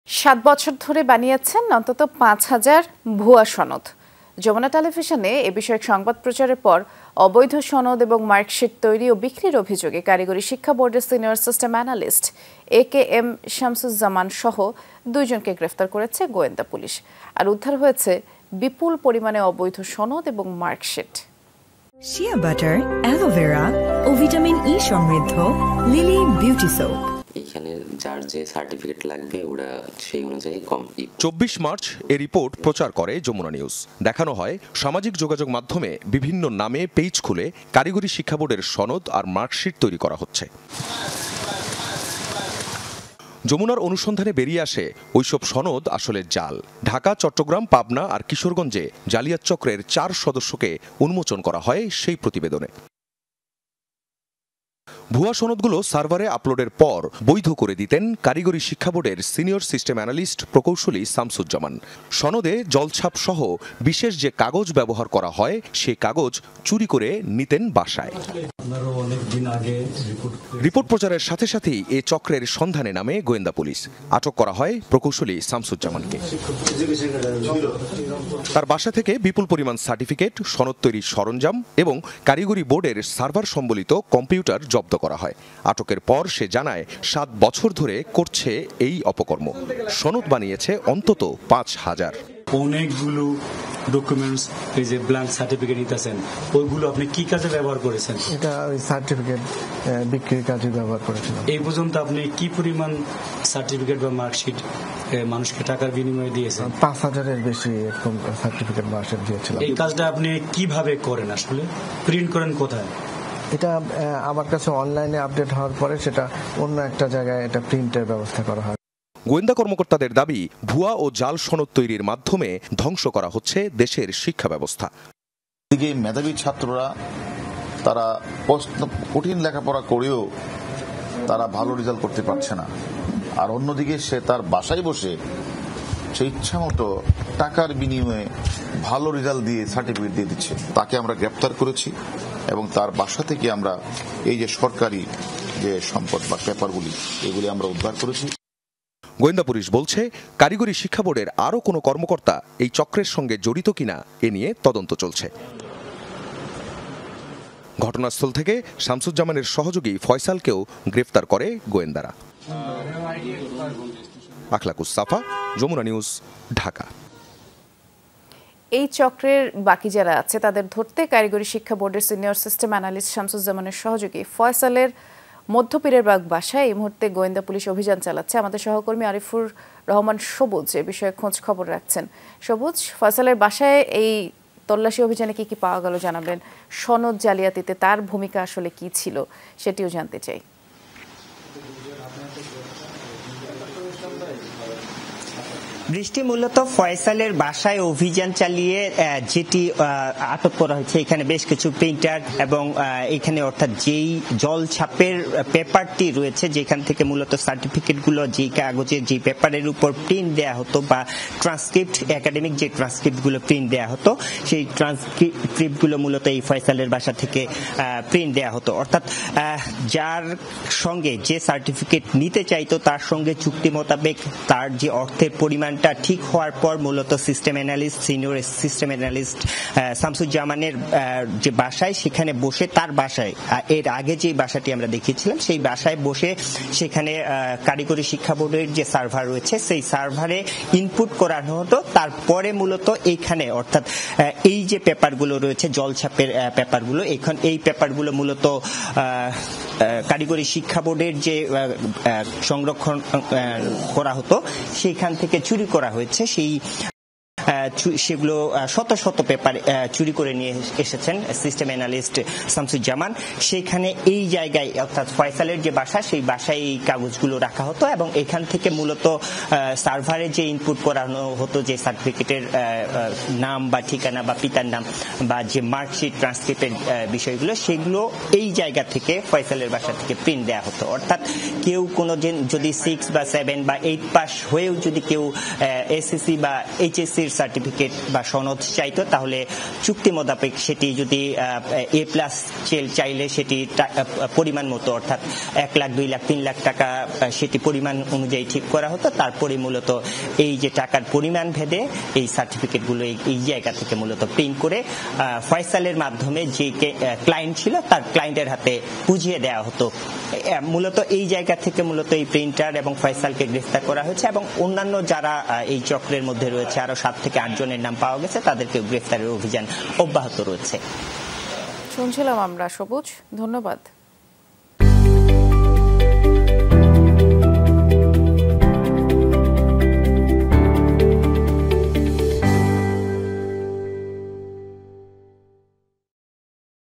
বানিয়েছেন ৫ হাজার ভুয়া সনদ, অবশেষে বন্দি হলেন ডিবির হাতে | Fake Certificate | Jamuna TV
সাত বছর ধরে বানিয়েছেন অন্তত ৫ হাজার ভুয়া সনদ। যমুনা টেলিভিশনে এ বিষয়ক সংবাদ প্রচারের পর, অবৈধ সনদ এবং মার্কশীট তৈরি ও বিক্রির অভিযোগে; কারিগরি শিক্ষা বোর্ডের সিনিয়র সিস্টেম অ্যানালিস্ট একে এম সামসুজ্জমান'সহ ২ জনকে গ্রেফতার করেছে গোয়েন্দা পুলিশ। উদ্ধার হয় বিপুল পরিমাণ অবৈধ সনদ এবং মার্কশীট।

Address:
Bangladesh.